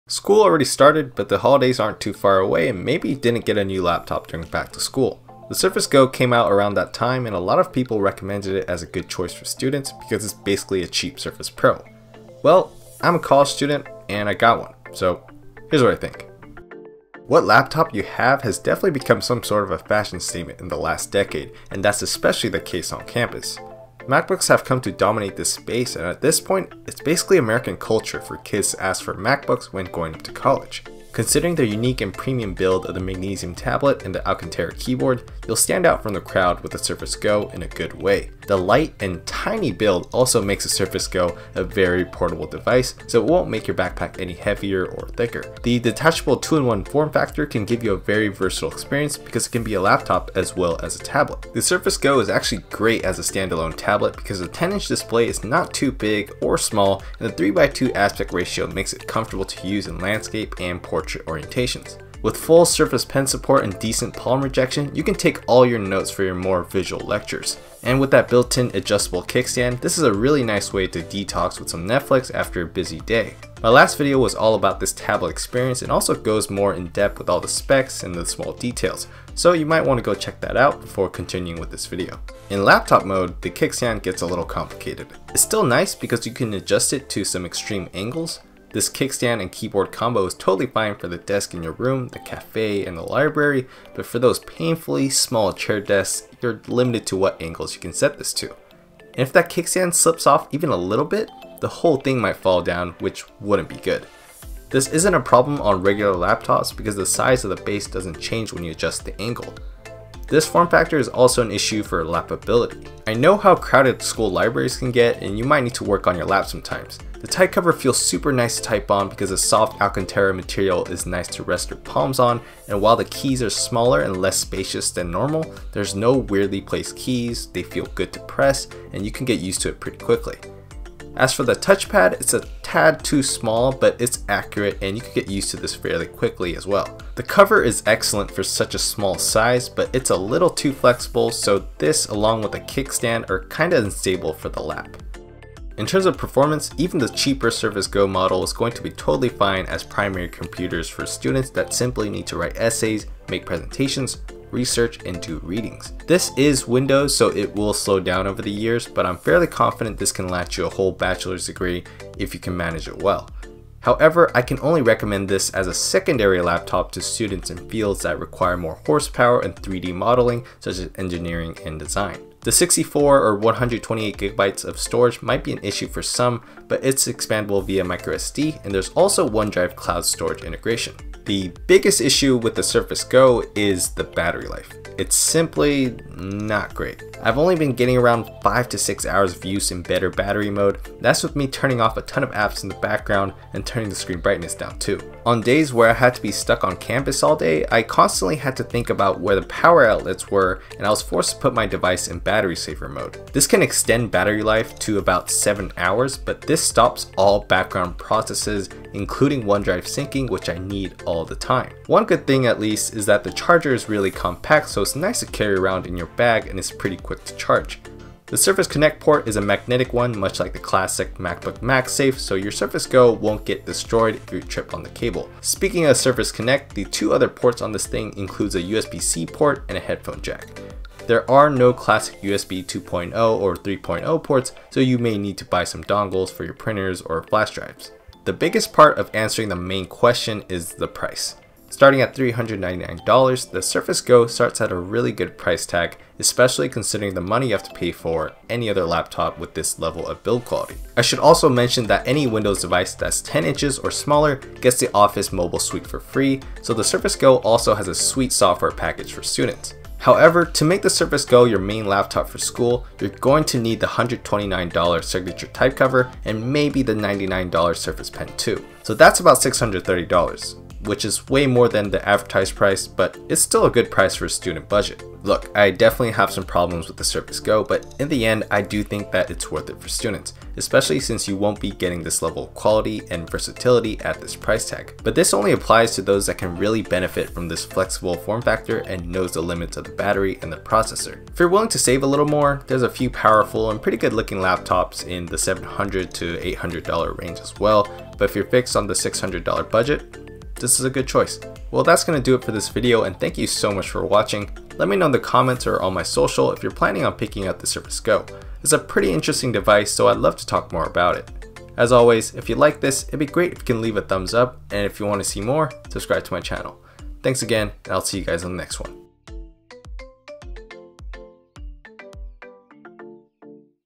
Is the Surface Go Worth it for Students? — A Student's Perspective
Microsoft Surface Go (Paid Link)

When the Surface Go launched, many tech journalists and influencers recommended the device as a good option for students, especially considering the affordable price tag starting at $399. Seeing that almost none of these people are actually students, I decided to try it out for myself at my university. Is it actually worth it? Find out in the video.